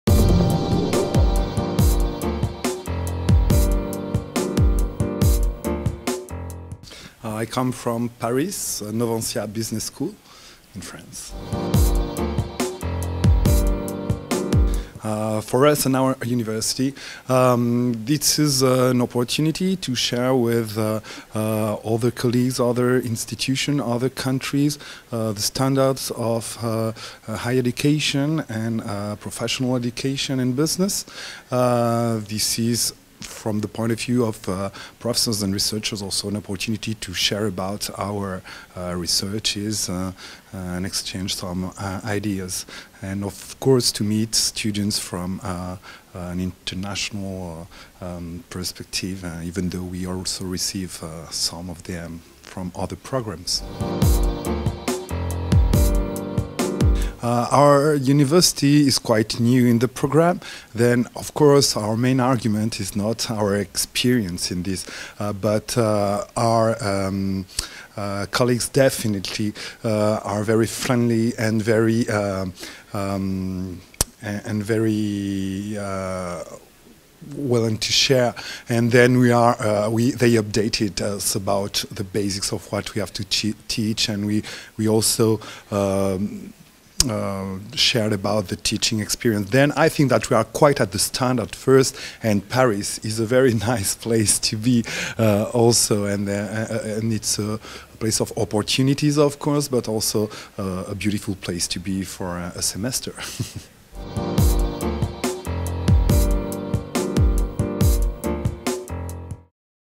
David Martin about the International Business School Alliance (IBSA)
The IBSA was established in 2003 and is a growing network of internationally oriented business schools offering a dual master award.
David Martin, Professor of Marketing, Novancia Business School Paris, explains the advantages.